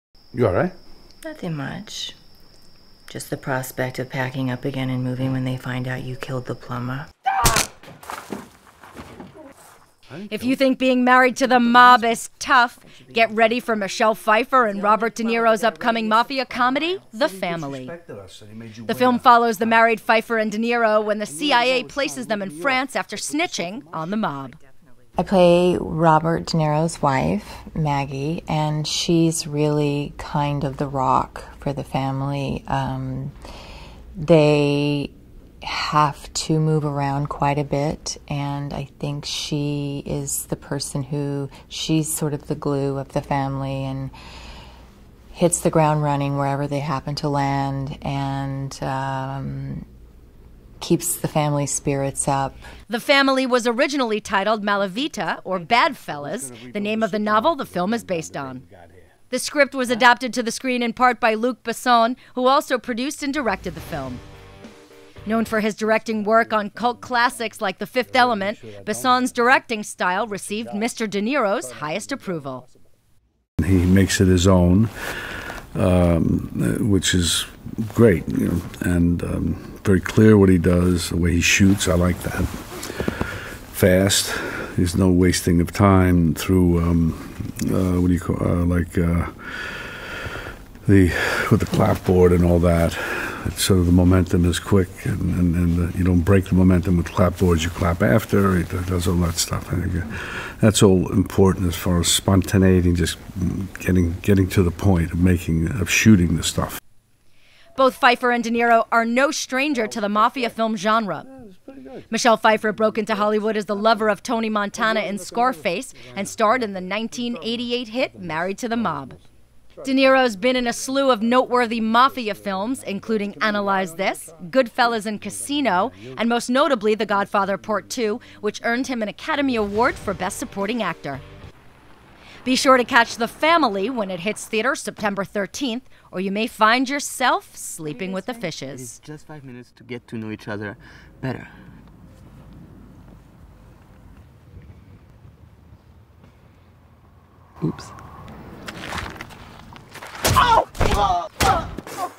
ROBERT DE NIRO, MICHELLE PFEIFFER DEAL WITH MOB FALL OUT
Robert De \Niro and Michelle Pfeiffer play a relocated husband and wife in the witness protection program after snitching on the mob as Michelle talks about her character and Robert talks about filming with director Luc Besson.